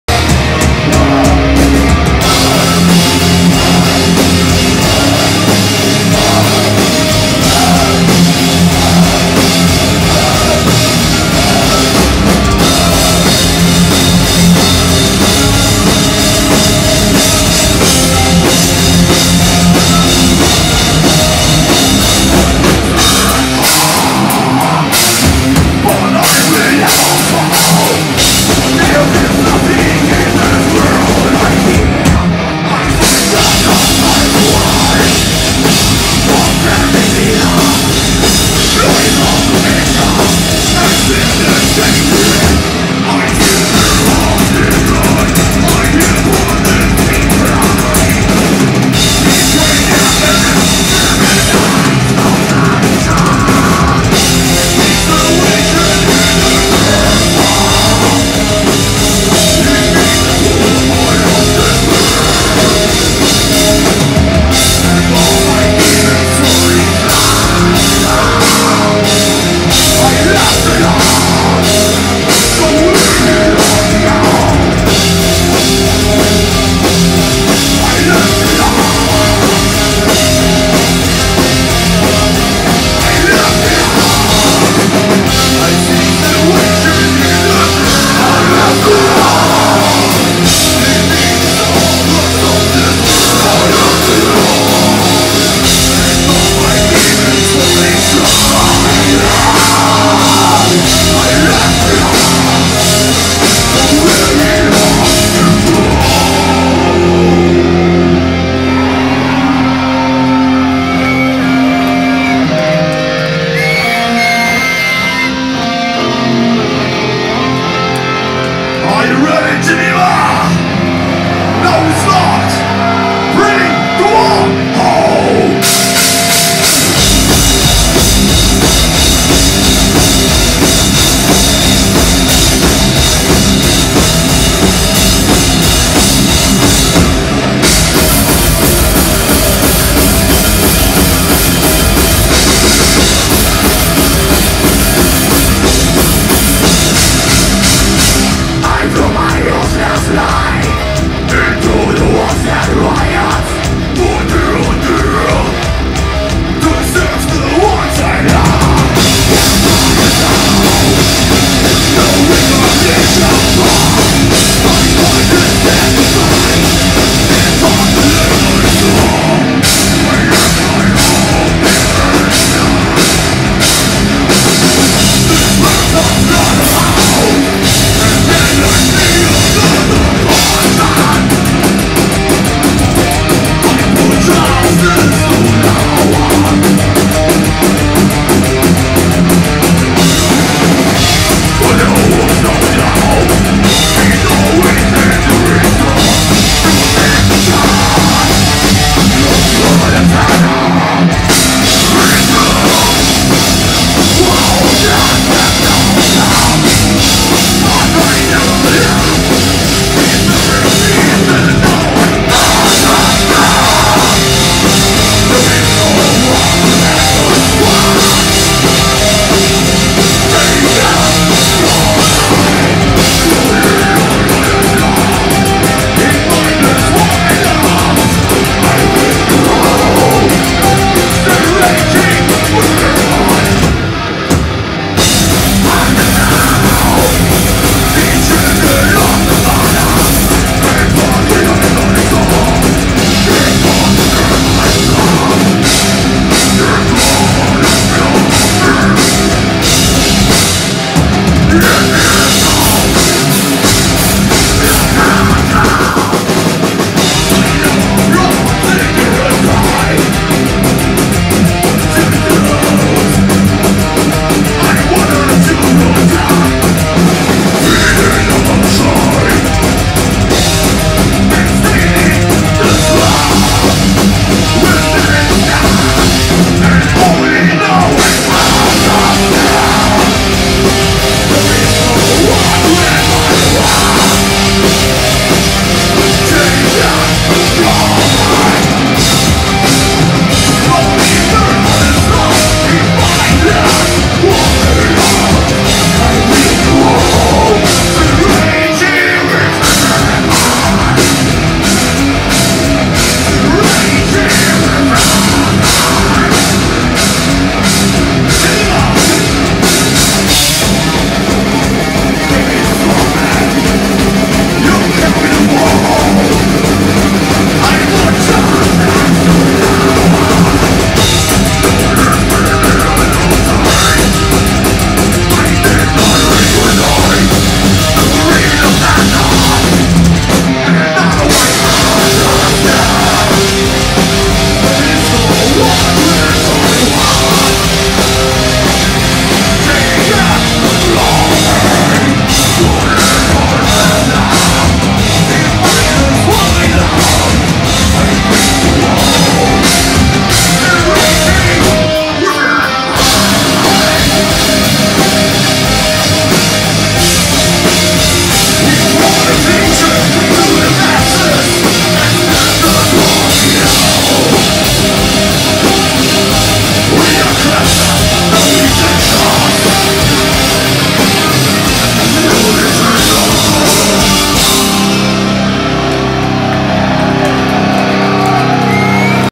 Heaven Shall Burn à l'Usine
Heaven Shall Burn, le 22.03.18 à l'Usine (PTR), filmé par Olive